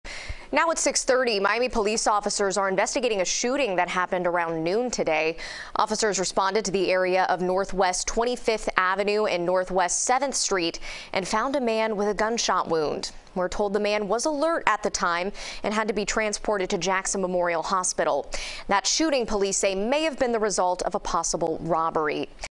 Miami PD Investigating Shooting That Could Stem From A Possible Robbery
Officers responded to the area of NW 25 Avenue and NW 7 Street where they found a man with a gunshot wound.